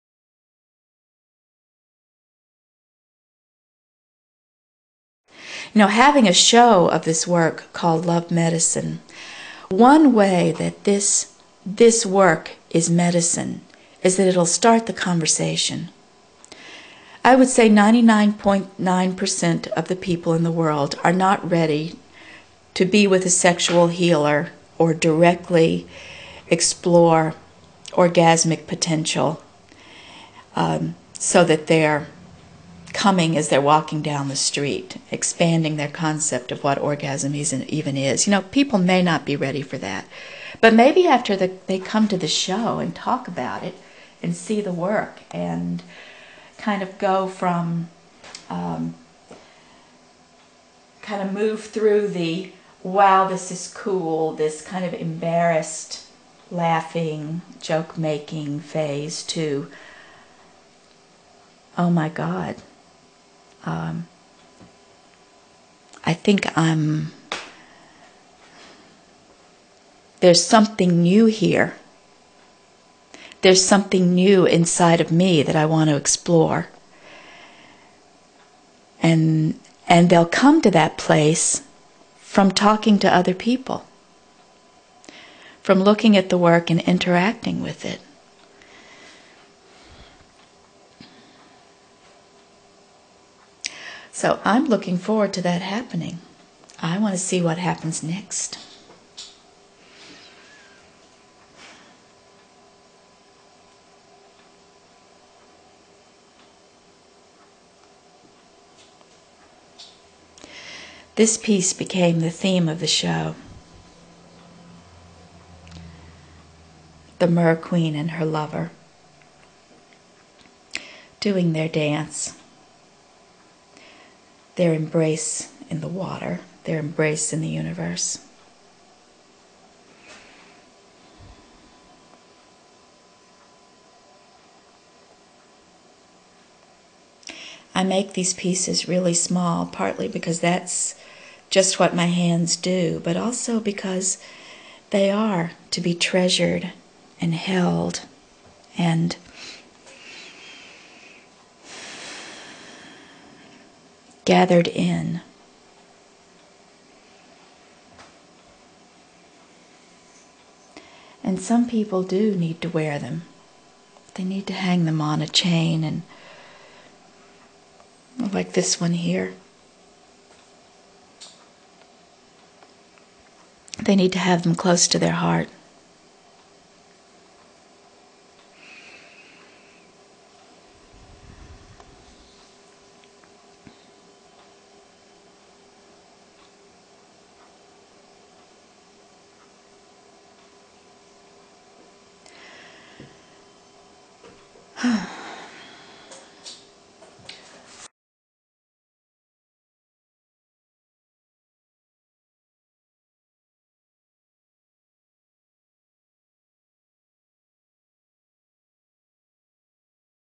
Love Medicine trailer
Carla of Touching Art, This video is taken from the 39 minute Love Medicine video, in which Carla talks about ways she hopes Love Medicine will promote orgasmic healing for people and for the whole planet.
The video was made by Bill Anderson of Go there to see more of his great videos.